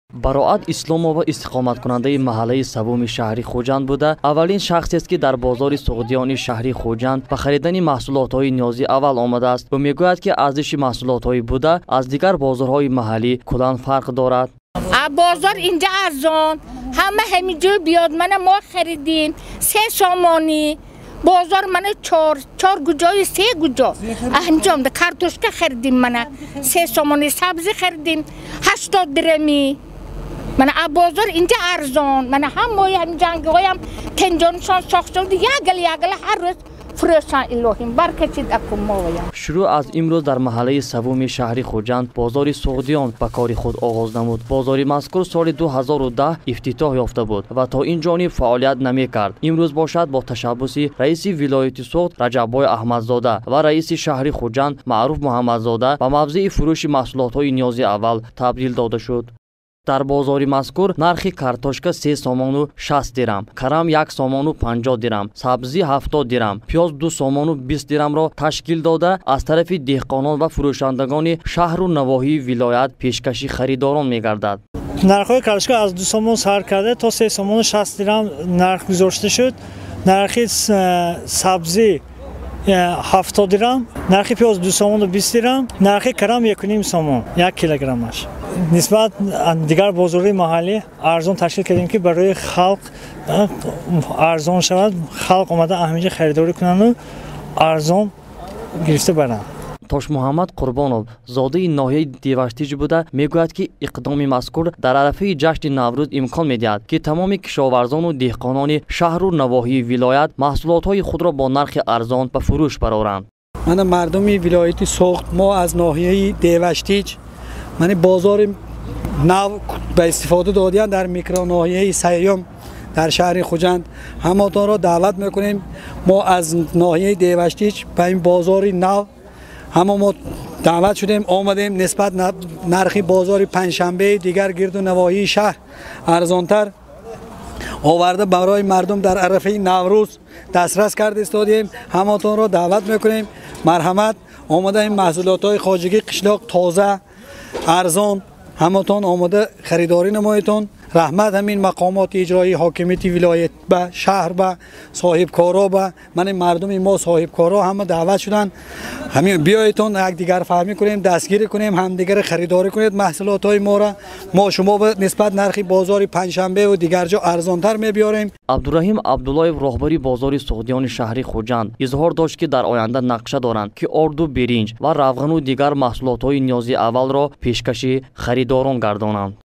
Паёми руз 10.03.2020 «Ташкили фурӯши маҳсулоти кишоварзӣ бо як қатор имтиёзот дар бозори "Суғдиён"»
Бо дастури рохбарияти вилояти Сугд бо мақсади фаровонии бозори истеъмолӣ ва ғани гардонии дастурхони сокинону меҳмонони шаҳри Хуҷанд бо мусоидати роҳбарияти шаҳр дар бозори Суғдиёни микроноҳияи 3-юм фурӯши густурдаи маҳсулоти кишоварзӣ аз дасти аввал роҳандозӣ карда шуд. Аз ҷониби маъмурияти бозор барои фурӯшандагон як қатор имтиёзҳо, аз ҷумла меҳмонхона, истгоҳ ва ҷойиҷораи комилан ройгон пешниҳод шудааст.